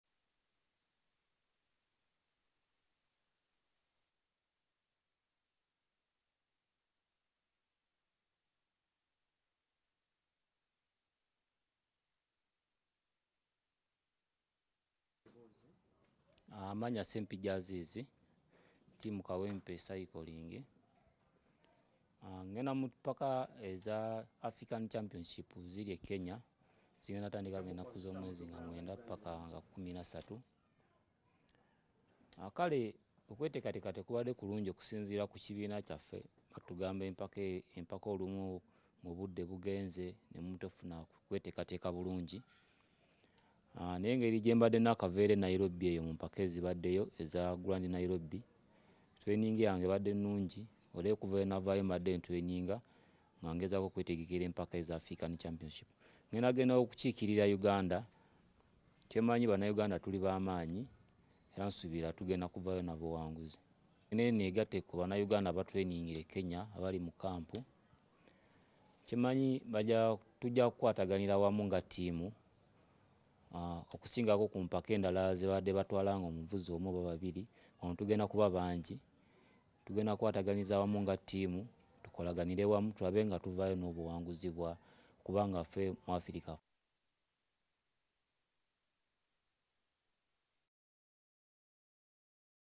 Obugaali bwempaka: Ssempijja Aziizi Awera, Agamba kuluno Empaka za Africa, Uganda yegenda oziwangula
Empaka za Africa ezokuvuga obugaali ezomwaka 2024 zakutegekebwa e Kenya omwezi guno waka w'enaku zomwenzi nga 09 ne 13 omwezi guno (October) e Kenya. Uganda yakukikirirwa abaguzi abasoba mu bataano era nga leero Aziizi Ssempijja asiibudwa mubutongole.